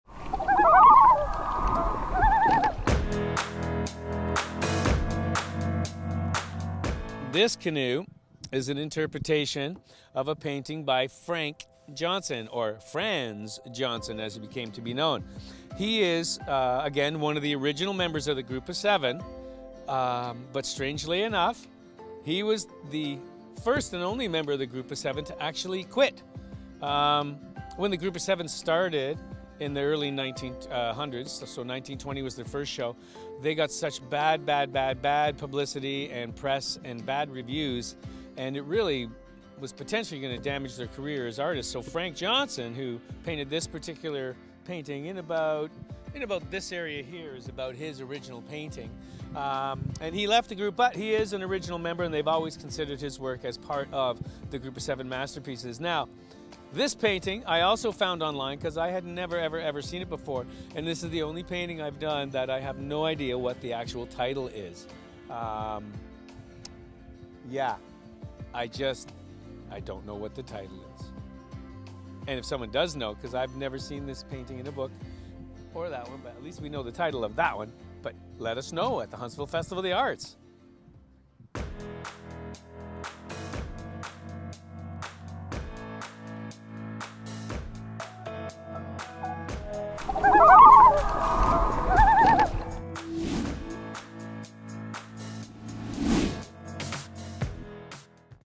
HfA Group of Seven Canoe Project - Frank Johnston
Artist Gerry Lantaigne tells us about his canoe inspired by AY Jackson. Created with Algonquin Outfitters and Limelight Muskoka.